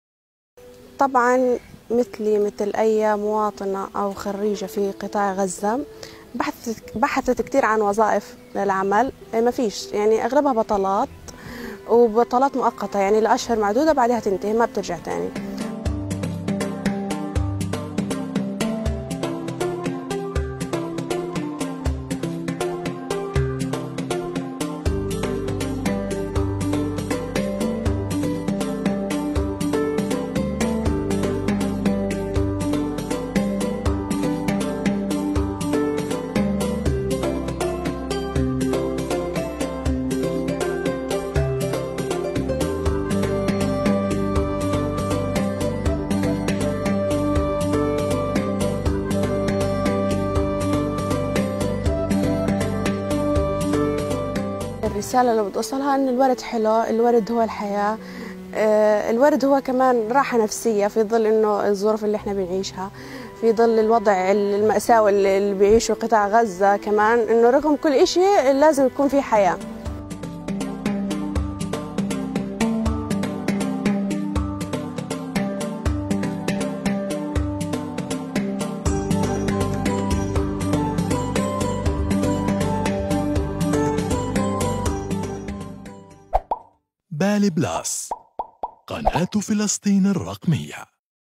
شاهد ممرضة فلسطينية تفتتح مشتل ورد خاص بها
هزمت البطالة وقررت أن تفتتح مشتل_ورود لتعتني بها وتقوم بتسويقها لكسب المال، تعرف عليها.